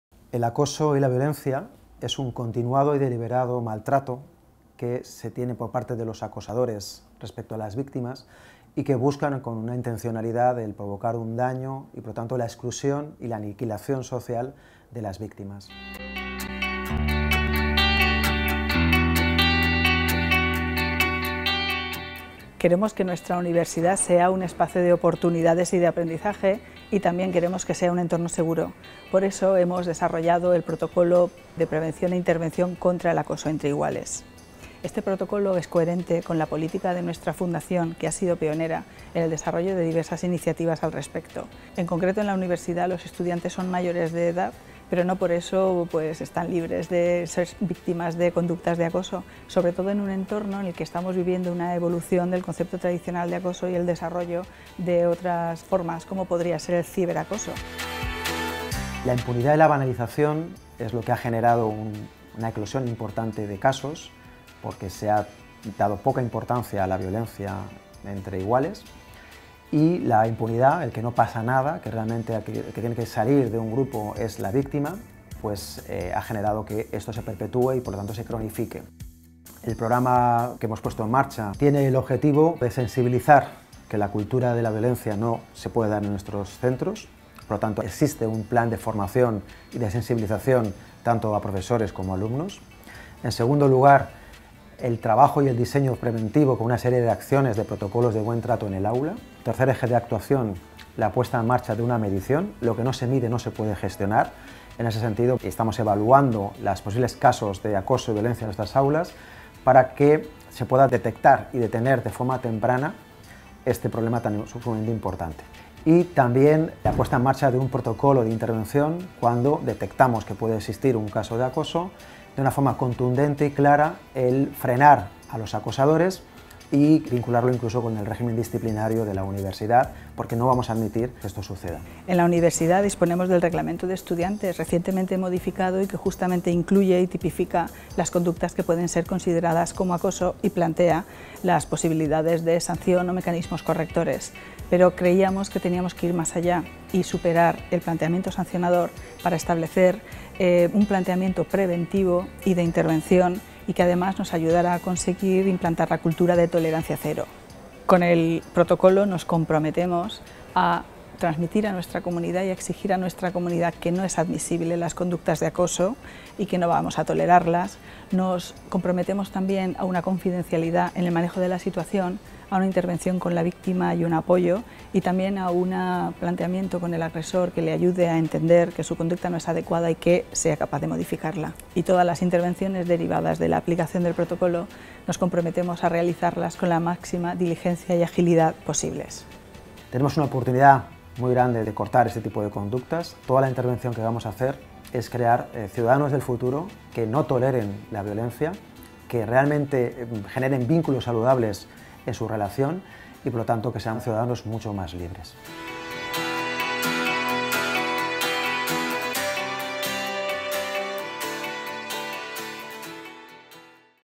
Protocolo de Prevención e Intervención contra el Acoso entre Iguales
Este Protocolo es coherente con la política de la Fundación Universitaria San Pablo CEU, que defiende fielmente los derechos fundamentales reconocidos en nuestra Constitución y en nuestro ordenamiento jurídico.
El Protocolo desarrollado en Universidad CEU Cardenal Herrera pretende alcanzar la finalidad de proteger y salvaguardar la integridad física y moral de todos nuestros estudiantes, así como su dignidad personal en un marco de convivencia pacífica, tolerancia y respeto.